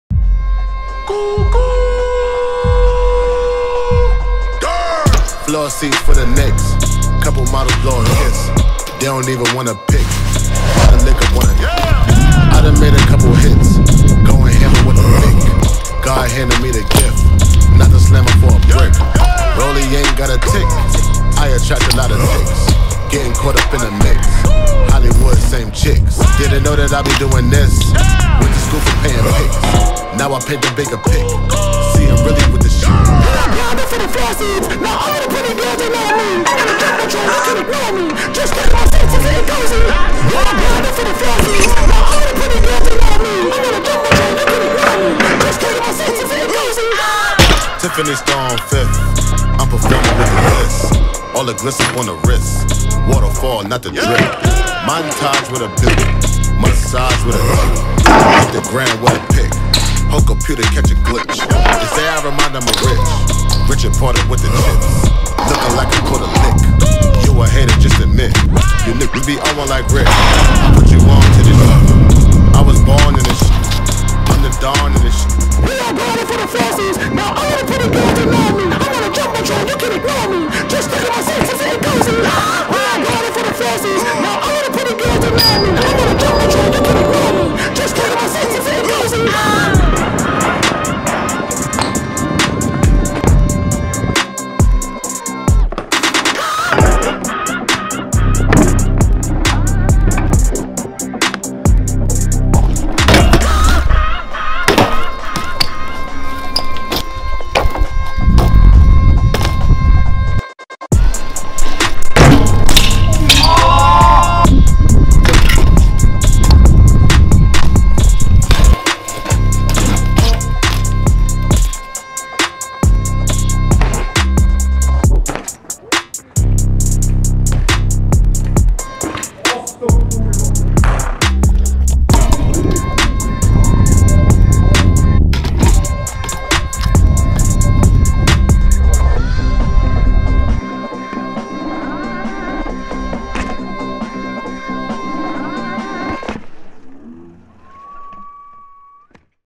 CODE X STRT LF 2020
Riders:

Márton Vajda, Jánó Kovács, Márton Fülöp, Bence Almássy, Tamás Gál, Márton Siklósi, Gergő Bene, Ádám Becsei, Farkas Olivér, Tibor Molnár, Tomi Kunecz, Tomi P Nagy

Filmed and edited by Domonkos Olescher